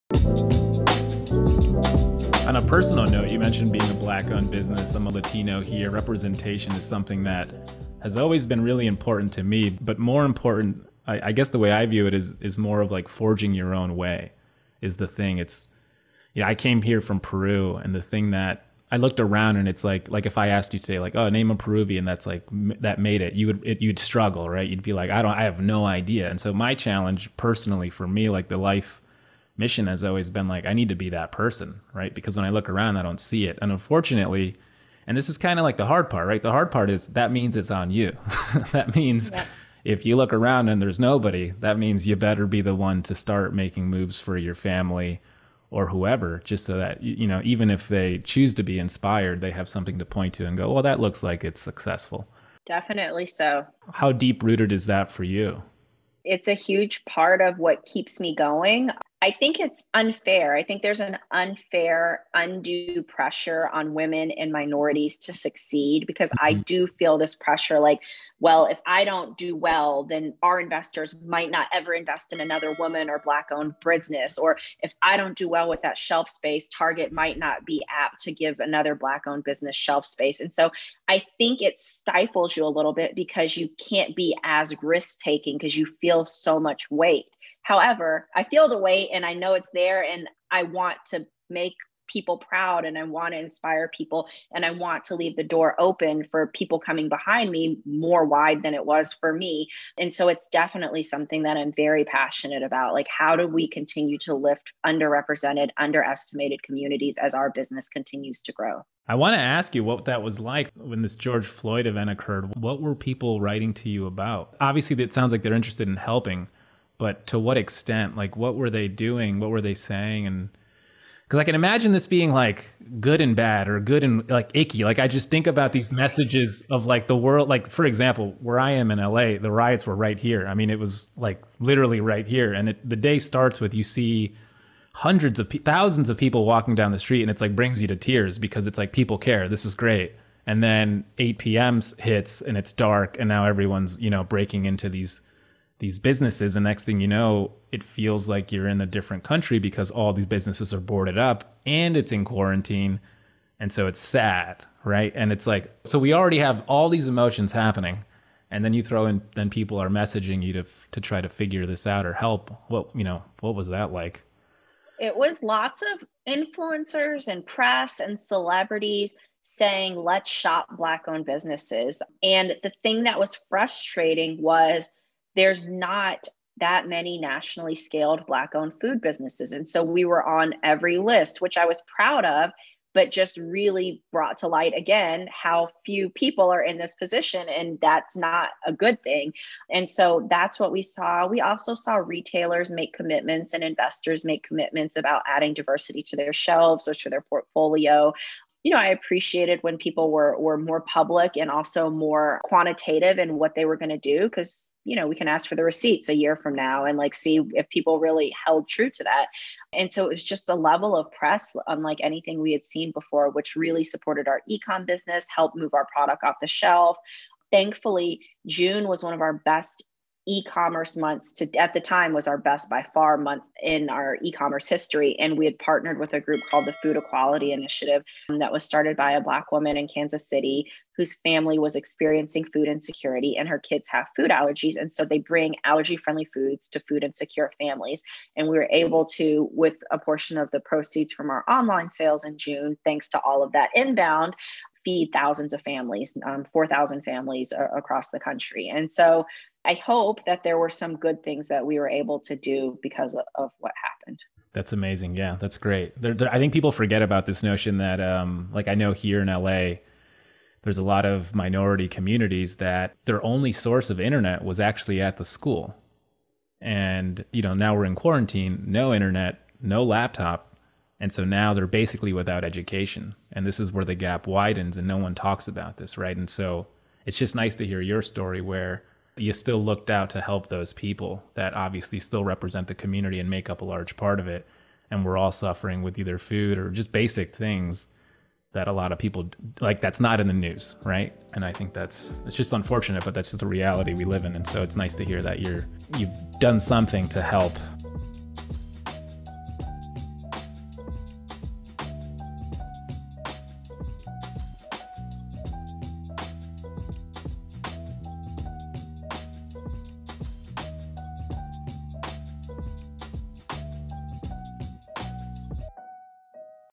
How Denise Woodard is a Role Model for Black Founders
Following the murder of George Floyd, there was a huge push to support black-owned businesses.  Denise Woodard, the founder of Partake Foods, said she saw her biggest spike in growth during the beginning of the summer of 2020.  However, this movement revealed a bigger problem: there just aren't enough black-owned businesses and entrepreneurs.  So, Denise Woodard has become a role model for many African American women who are looking to take the leap into entrepreneurship.

In this clip of our podcast, we wanted to ask Denise Woodard what it has been like acting as a role model for so many.  This is an inspirational clip you won't want to miss.